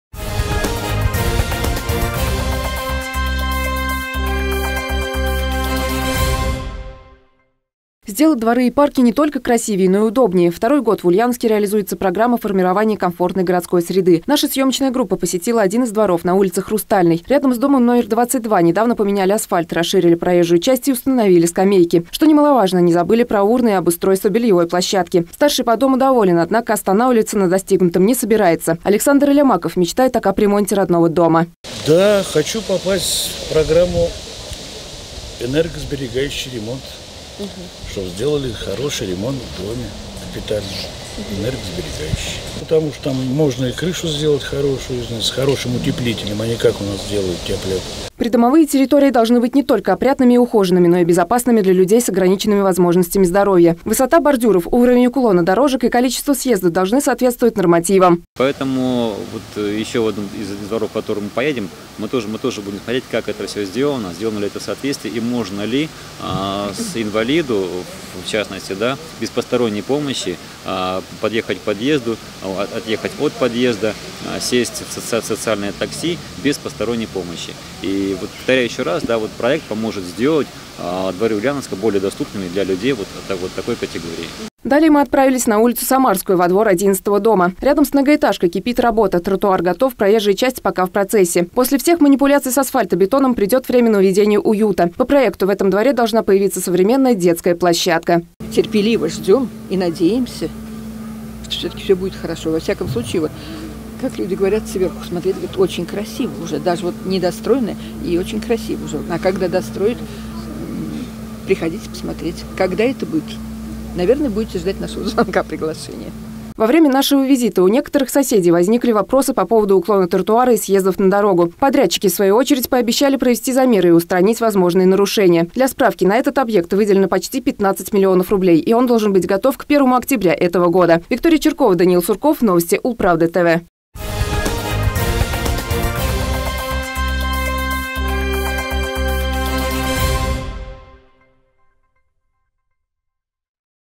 Под наклоном и правильной высоты. Ульяновские дворы начали ремонтировать по нормативам
Рядом с домом №22 на Хрустальной недавно поменяли асфальт, расширили проезжую часть и установили скамейки.